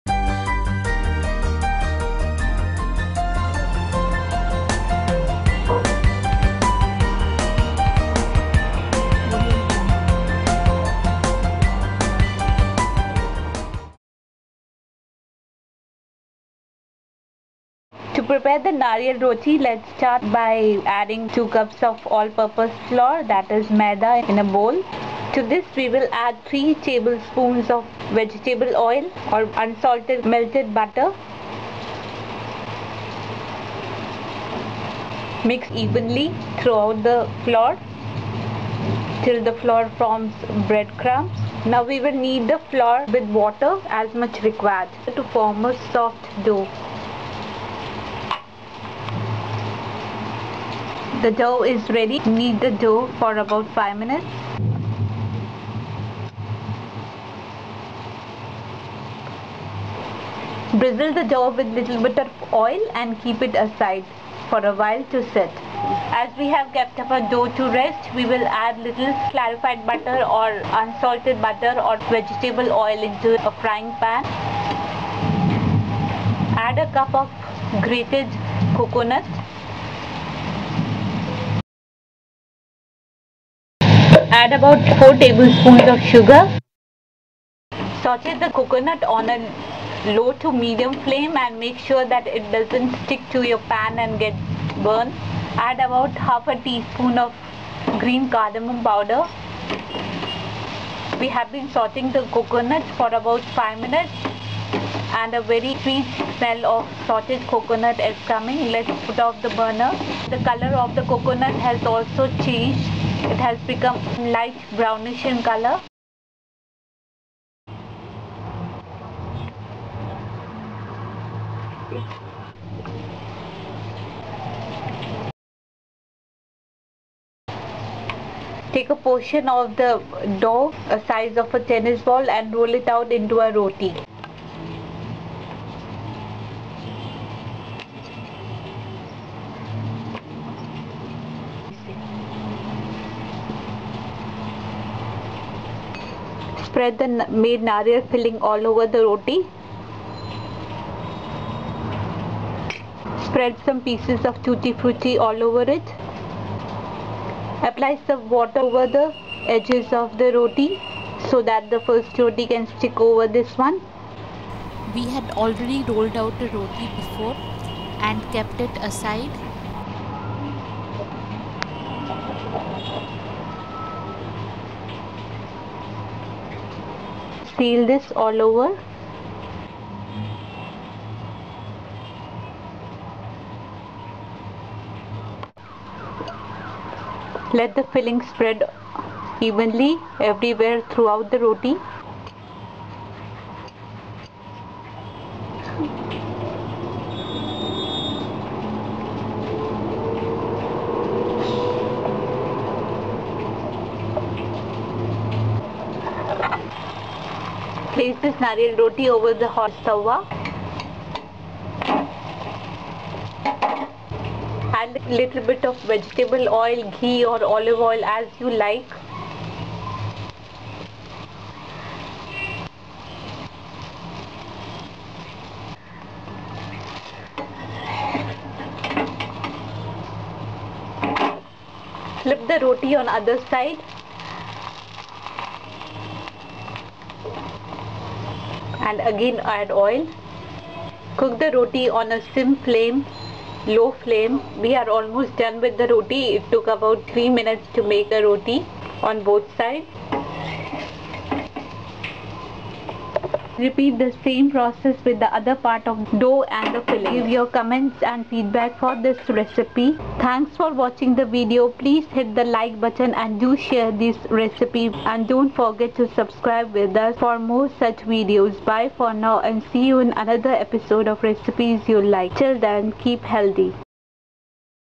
Sweet Nariyal Roti
Coconut  Indian Bread (Nariyal Roti )This is like a quick sweet snack or can be your healthy breakfast or you can pack this for your kids lunchbox

We prepare that you can make and enjoy. You can send in your requests to us for food, health and living.
Share and Recommend this yummy recipe.

OR